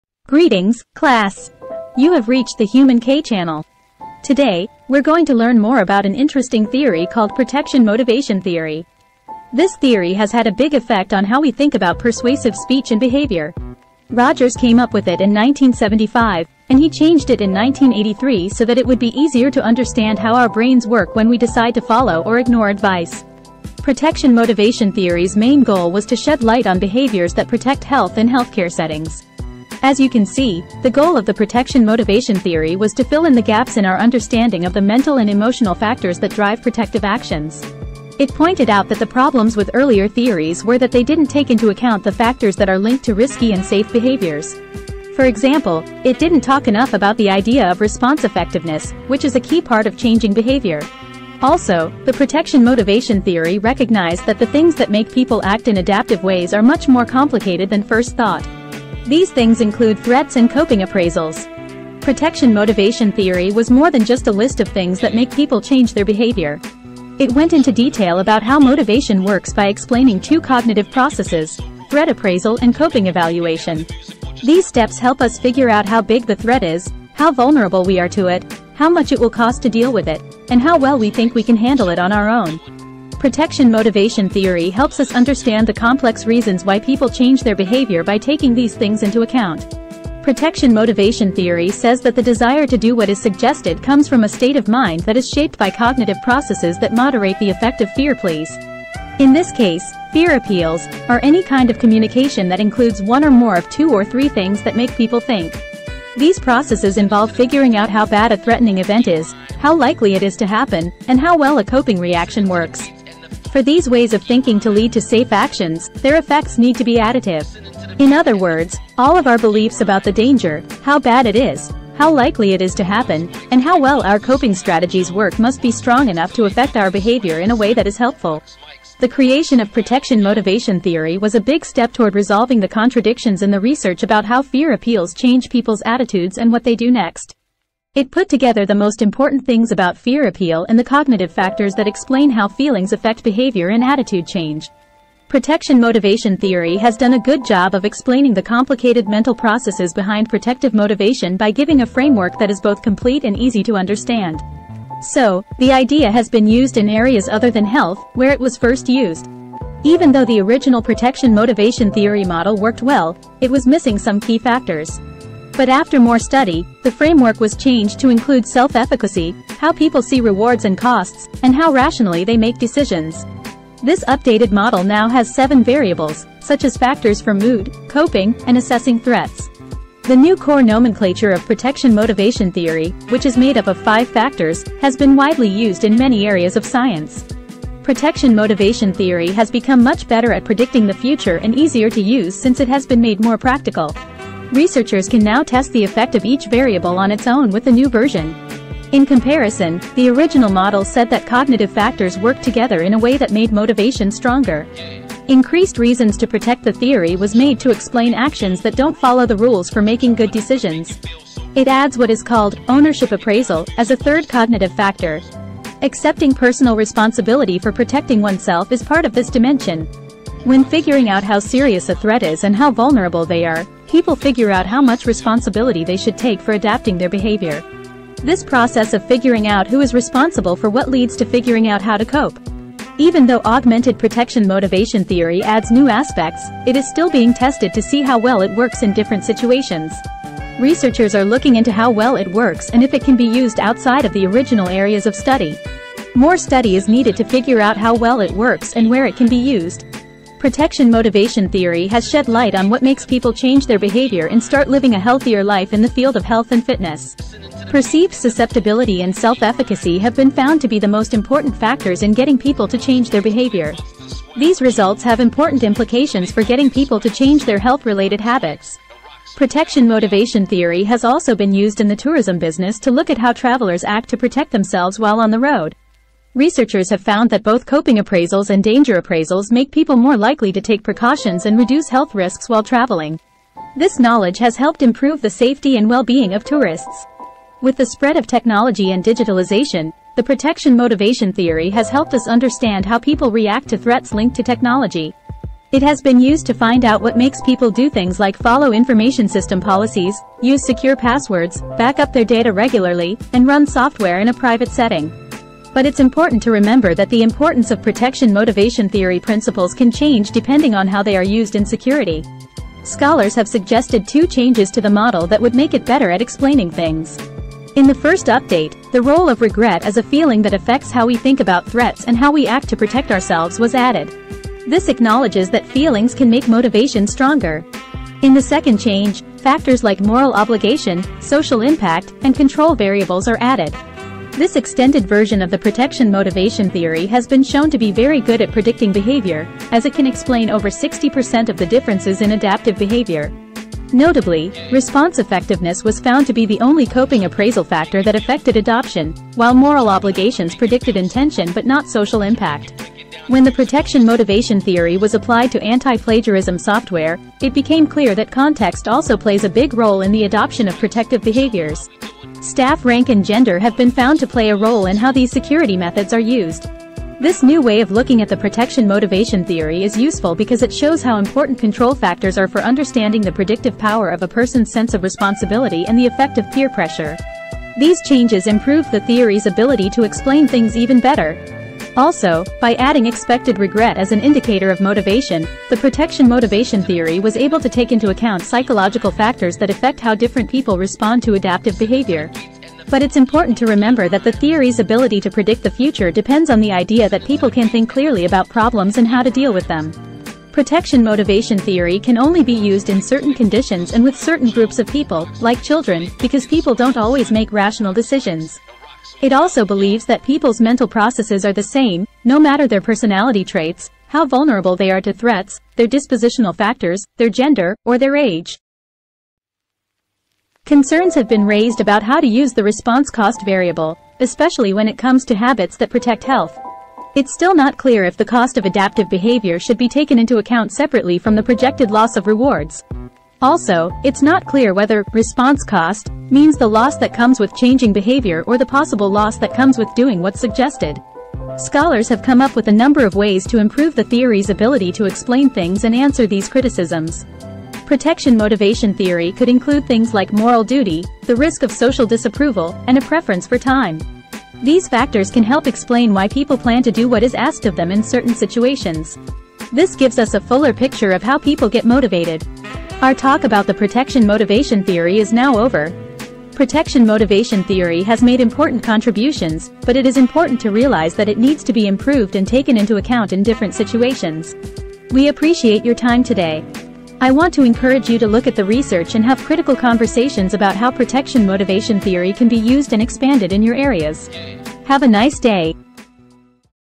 Understanding Protection Motivation Theory
Rogers came up with Protection Motivation Theory (PMT) in 1975 and changed it in 1983 to explain how effective communication can change people's behavior. Protective measures like helmets, physical exams, and living a healthy lifestyle can keep people from getting hurt, but people don't always use them.